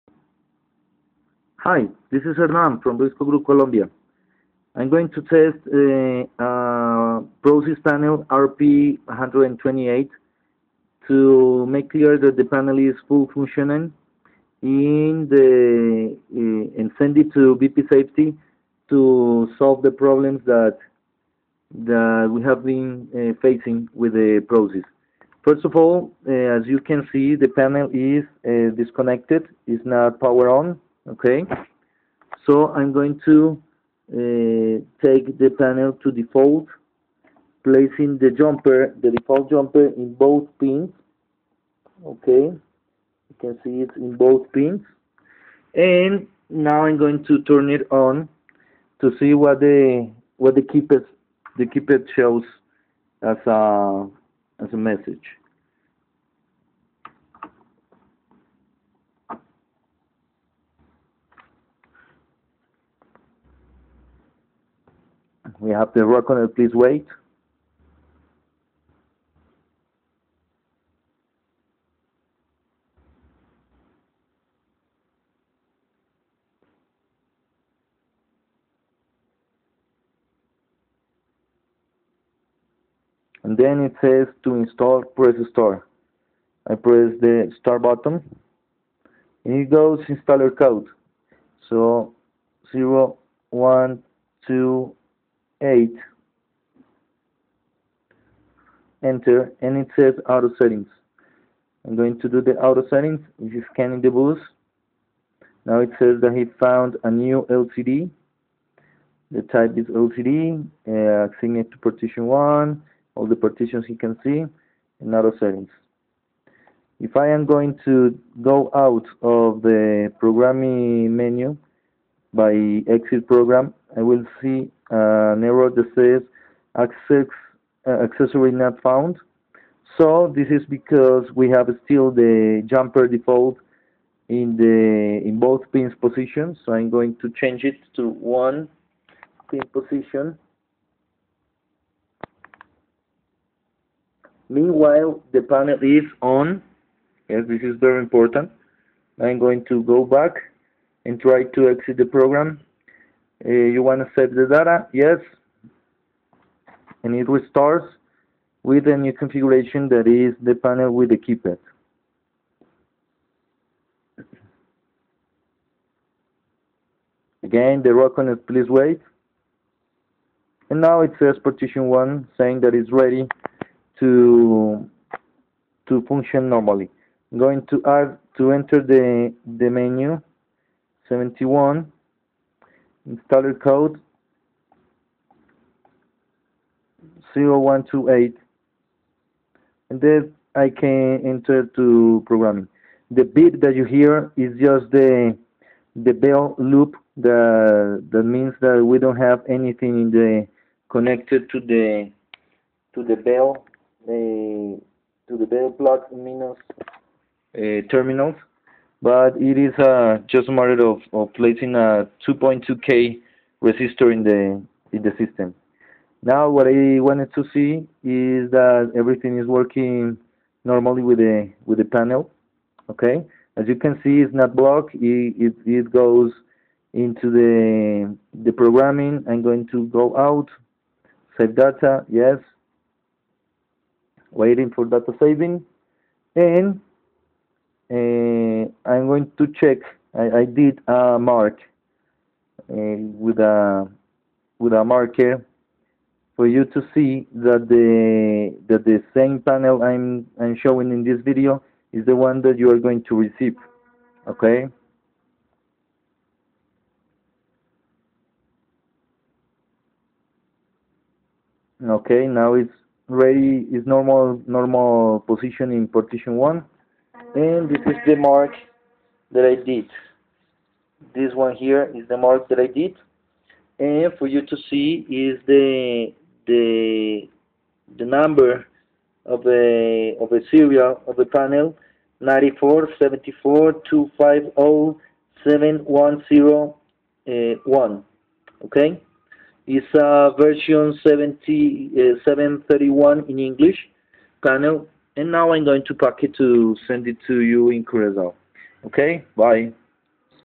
ProSYS startup Verification
Verification of the normal functioning of a ProSYS panel